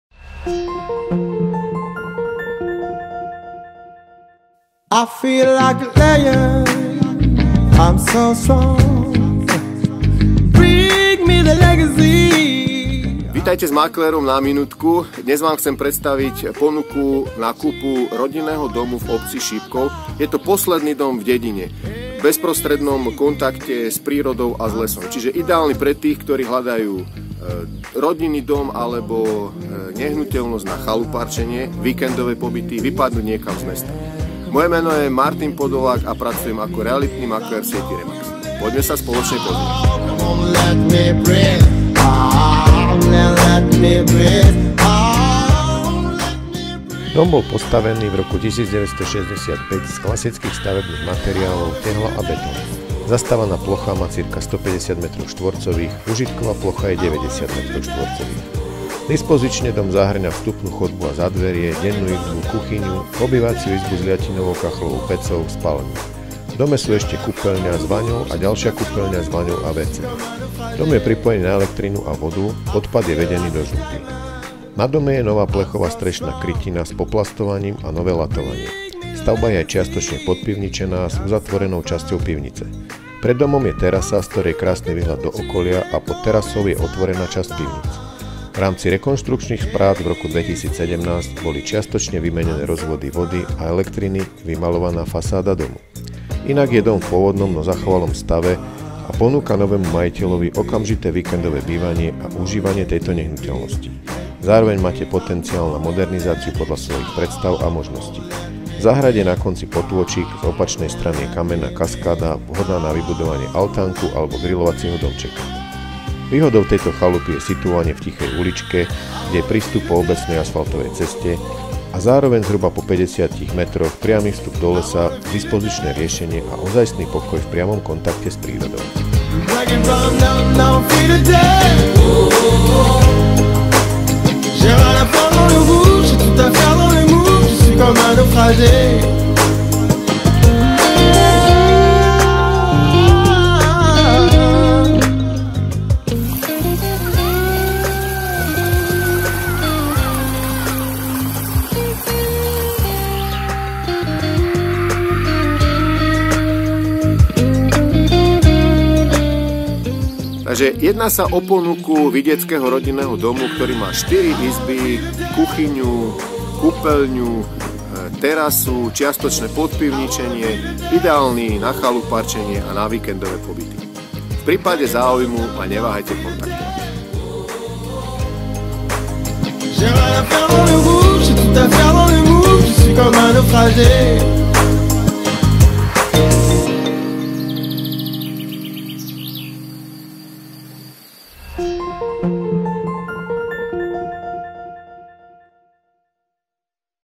dom šipkov prehliadka
Dnešný spôsob rýchleho života prináša ľuďom v mestách potrebu aktívneho relaxu a odpočinku , najlepšie na vidieku , bezprostredne v kontakte s prírodou a peknou krajinou.

Táto ponuka je smerovaná presne pre Vás, ktorí hľadajú nehnuteľnosť za účelom relaxu , v lone prírody.
Samotný dom sa nachádza v malebnej obci Šípkov , v okr. Bánovce nad Bebravou. Urččite oceníte , že z okolitých miest to nebudete mať ďaleko , a ideálna dostupnosť pre chalupárov ja takáto :
Prievidza : 55km a 60min cestou autom.
Trenčín : 33 km a 37 min
Partizánske : 42km a 45min
Bánovce nad Bebravou : 21km a 25min autom.

Prístup až priamo k domu je po asfaltovej ceste ktorá patrí obci.

Dom je situovaný na konci dediny, doslova posledná stavba v dedine. Máte krásny výhľad na okolie a cestičku do lesa , ktorá vedie popri dome. Pozemok o výmere 803m2 je časť vo výlučnom vlastníctve a časť v podielovom , v miernom svahu je čiastočne zastavaná plocha a čiastočne záhrada. V záhrade je na konci potôčik , z opačnej strany ja kamenná kaskáda vhodná na vybudovanie altánku alebo grilovacieho domčeka.

Dom bol postavený v r. 1965 z klasických stavebných materiálov , tehla a betón. Zastavaná plocha má cca 150m2 , úžitková plocha je 90m2. Dispozične dom zahrňa vstup a zádverie, dennú izbu , kuchyňu , obývaciu izbu s liatinovou kachlovou pecou , spálňu. V dome sú ešte kúpeľňa s vaňou a ďaľšia kúpeľňa s vaňou a wc. Dom je pripojený na elektrinu a vodu , odpad je vedený do žumpy. Na dome je nová plechová strešná krytina s poplastovaním / r.2017 /. Dom je aj čiastočne podpivničený s uzatvorenou časťou pivnice, ako aj otvorenou - pod terasou. V minulosti bol vykurovaný aj ústredne - radiátormi , ktoré sú momentálne odpojené a systém vypustený.V súčasnosti je kúrenie na pevné palivo .
Pred domom je terasa s novou izoláciou a oplotením - r.2017  / treba dokončiť položenie dlažby / s parkovacím miestom pre auto ,alebo terasové sedenie. V rámci rekonštrukčných prác v r.2017 boli čiastočne vymenené rozvody vody a elektriny, vymaľovaná fasáda domu.
Inak je dom v pôvodnom, no zachovalom stave a ponúka novému majiteľovi okamžité víkendové bývanie a užívanie tejto nehnuteľnosti. Zároveň máte potenciál na modernizáciu podľa svojich predstáv a možností.
Takže kúpou získate dom  - chalupu na trávenie voľnočasových aktivít či už priamo v Šípkove , na svojom pozemku , prechádzkami a hubárčením po okolí , cykloturistikou , výletami za koňmi do Motešíc , alebo za kúpaním do Trenčianskych Teplíc / 16km /

Kúpou je dom voľný IHNEĎ. Financovanie je možné cez HU , alebo vlastné prostriedky.
V cene sú zahrnuté  poplatky spojené s prevodom nehnuteľnosti , vypracovanie kúpnych zmlúv a polplatky na katastrálny servis.
Pre bližšie informácie a termín na obhliadku ma neváhajte kontaktovať.